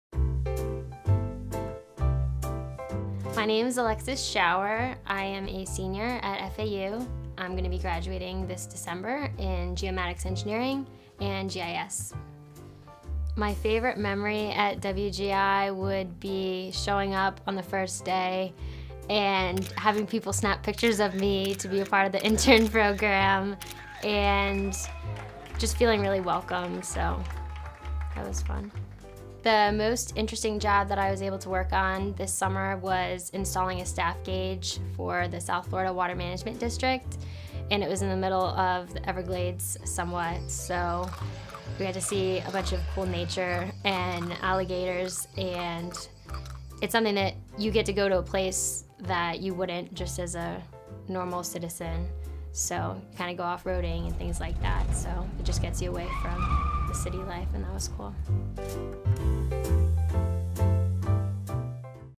WGI Internship
Alexis Schauer talks about her time spend as an intern at WGI.